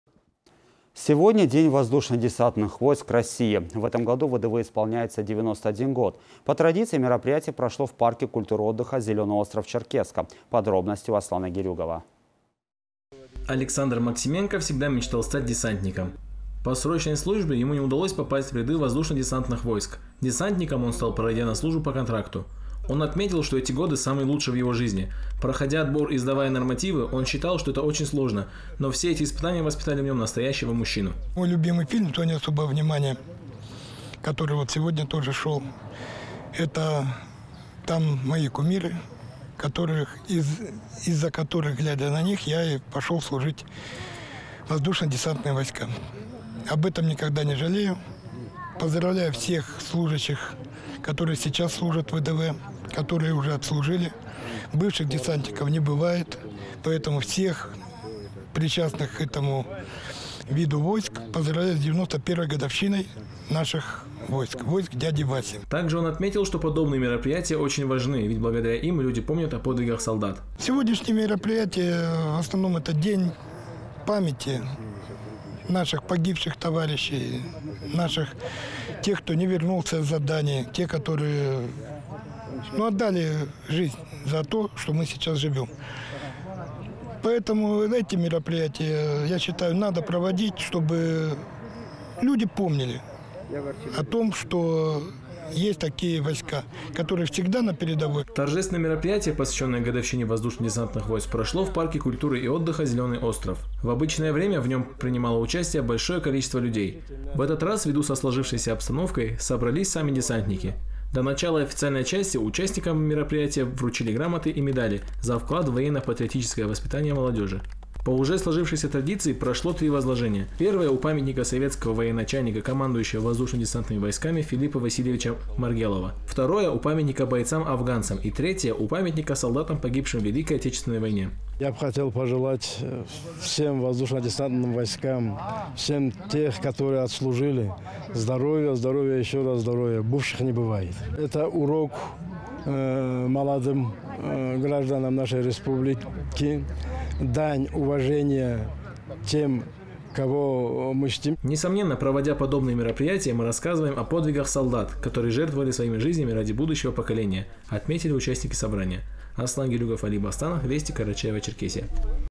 2 августа День воздушно-десантных войск России
2 августа День воздушно-десантных войск России. В этом году ВДВ исполняется 91 год. По традиции мероприятие прошло в парке культуры и отдыха "Зеленый остров" Черкесска